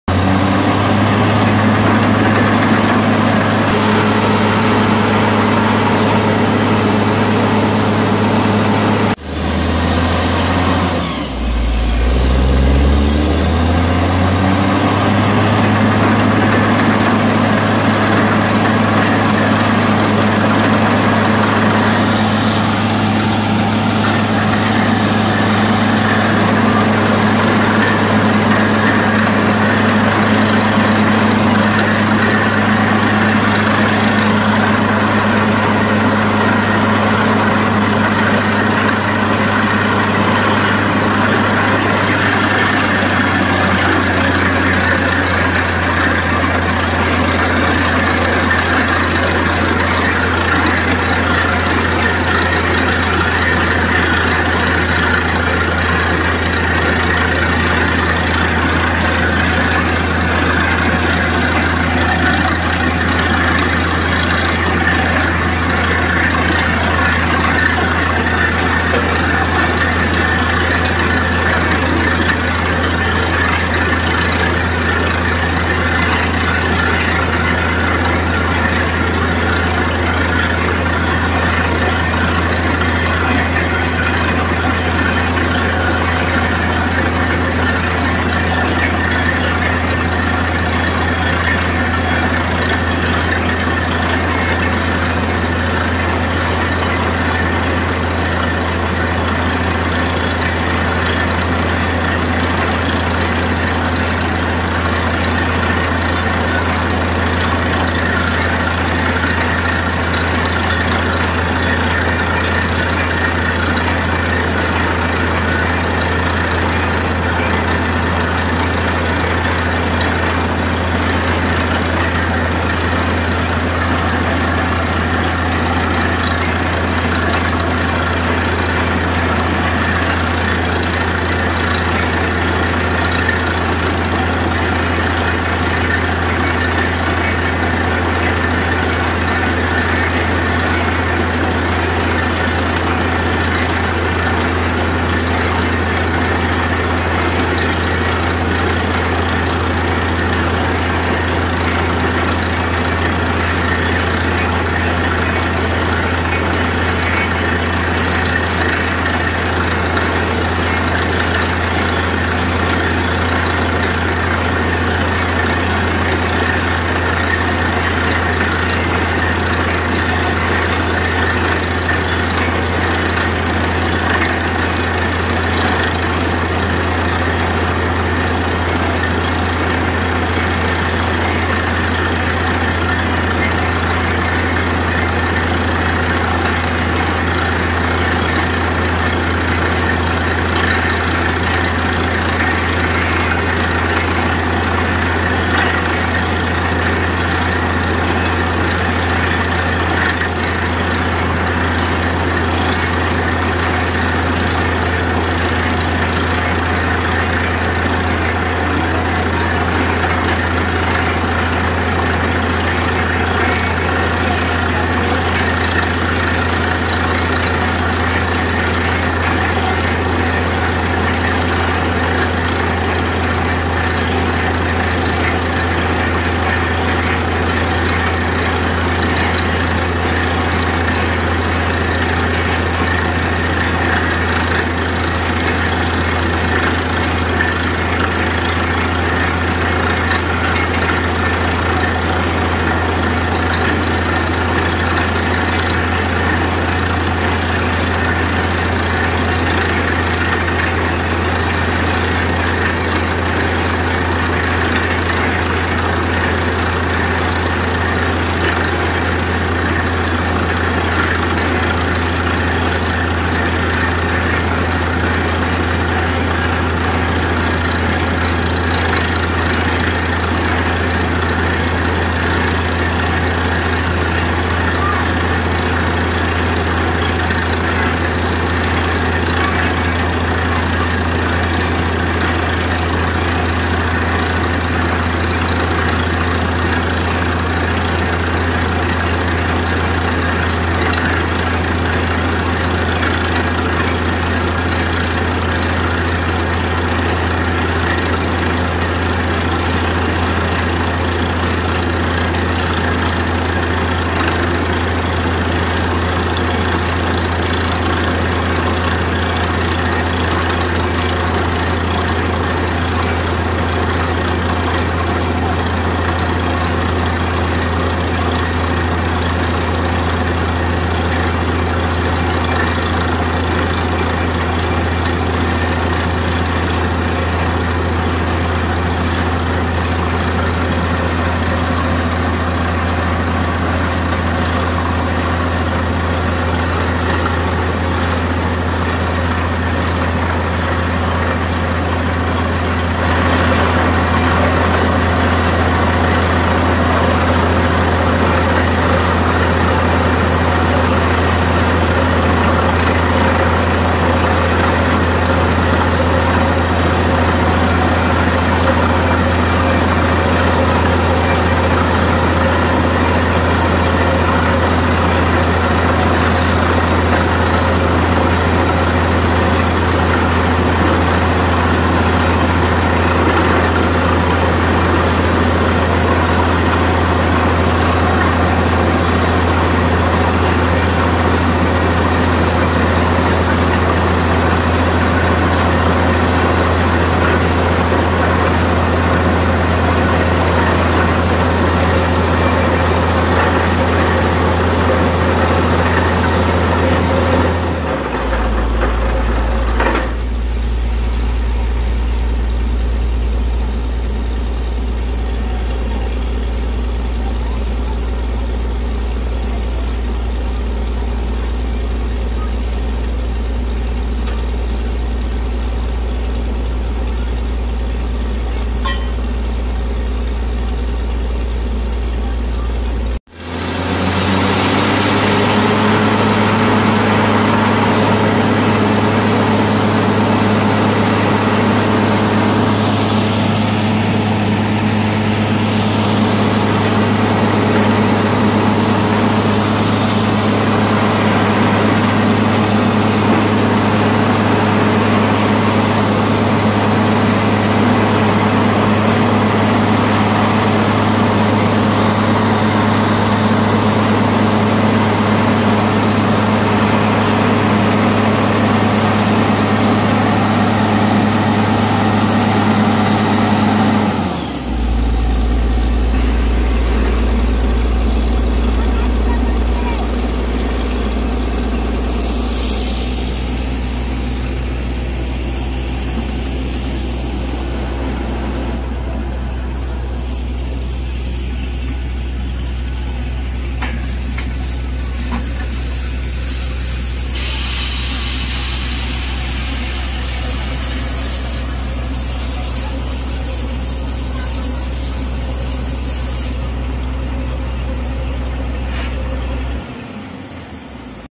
Borewell Drilling 20 HP Motor with Coconut Water checking Method | 110 फिट बोरवेल खुदाई का वीडियो
Borewell Drilling 20 HP Motor with Coconut Water checking Methods | Water checking for Boring
Borewell Drilling 100% पानी with Coconut Water checking Methods